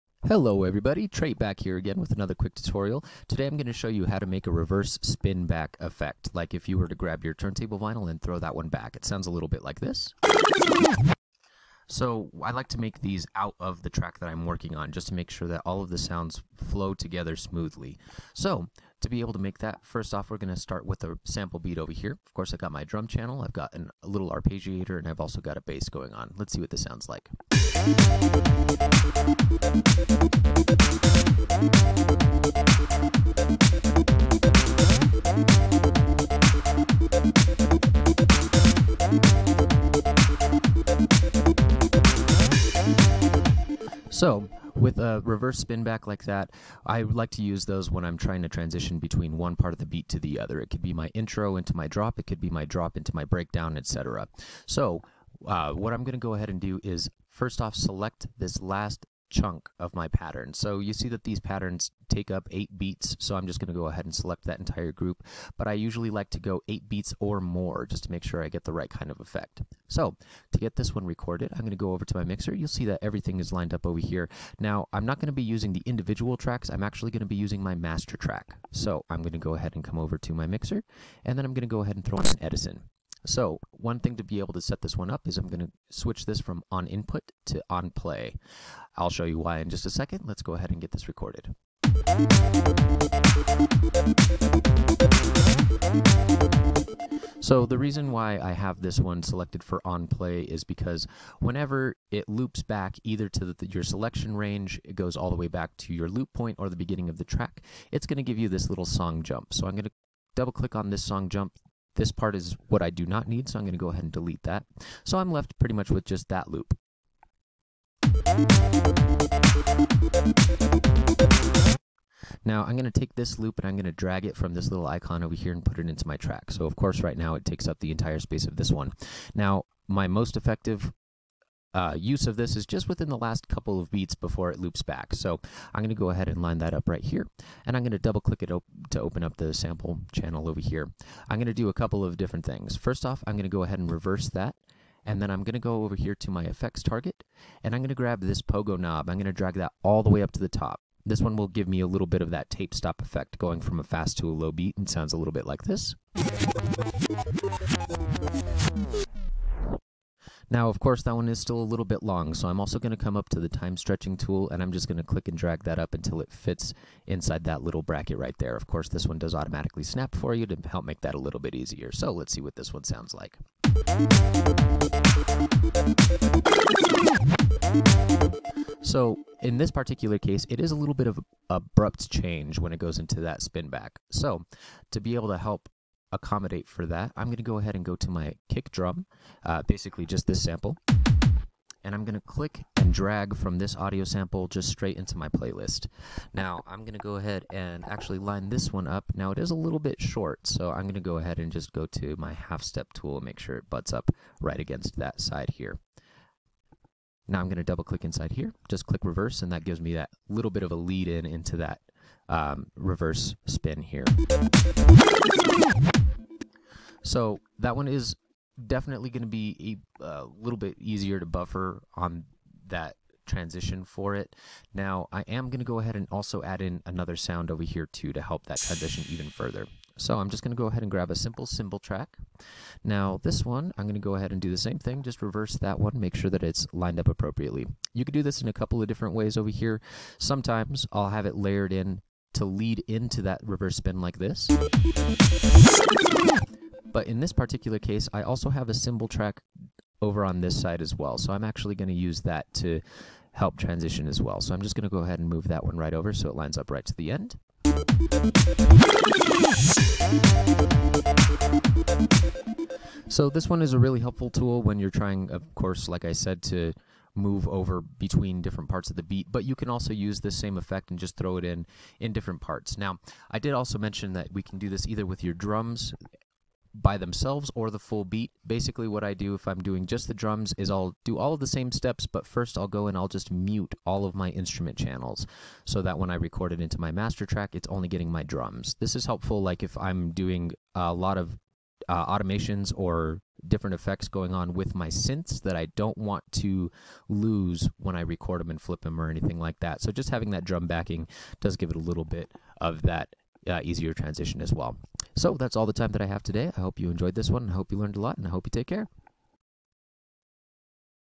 5 min FL Reverse Spinback Tutorial
Hello everyone!
This tutorial will show you how to emulate a turntable spinback using Edison inside FL Studio!